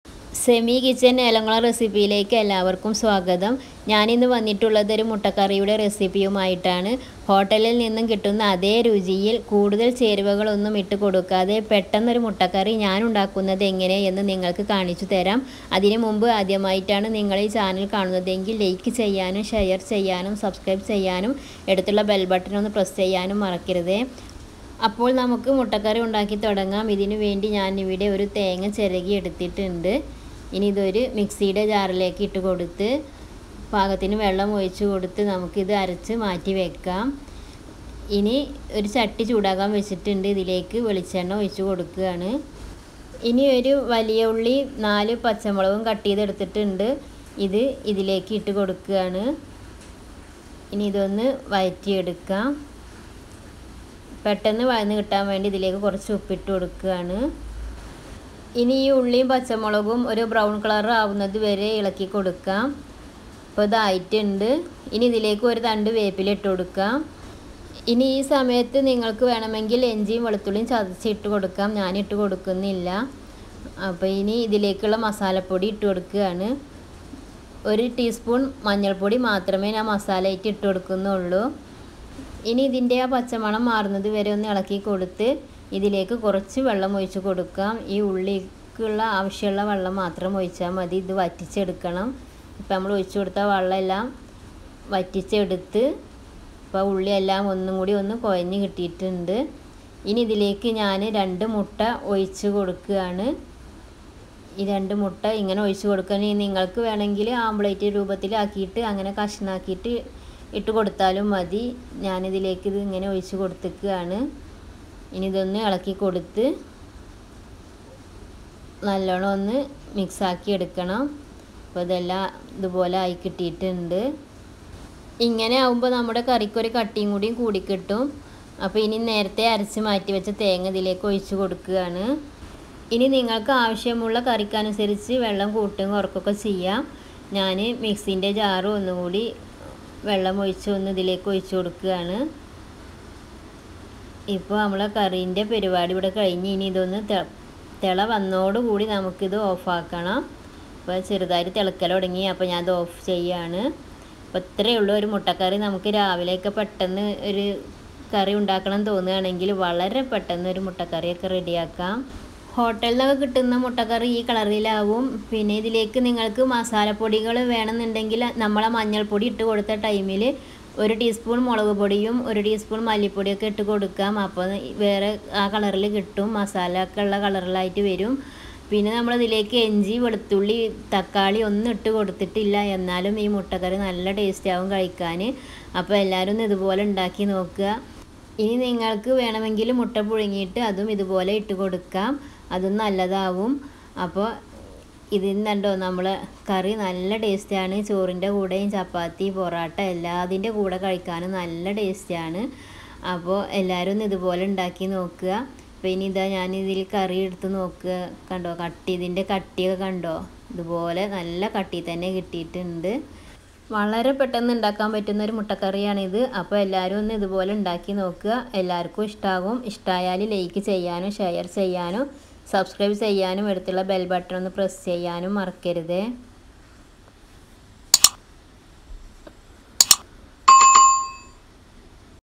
Mutta kari recipe in Malayalam/മുട്ടക്കറി/Egg curry Semi kitchen Elamkulam recipe/2021
Egg curry/Recipe in Malayalam
ചാപ്പാത്തി പൊറാട്ട ഇവയുടെ കൂടെ ഒഴിച്ച് കറിയായി പെട്ടെന്ന് ഉണ്ടാക്കാൻ പറ്റുന്ന മുട്ടക്കറി റെസിപ്പി

Happy cooking with semi kitchen Elamkulam recipe

Friends, please do share your feedback comment section below, Thanks you so much

Happy cooking

2021
Malappuram
Kerala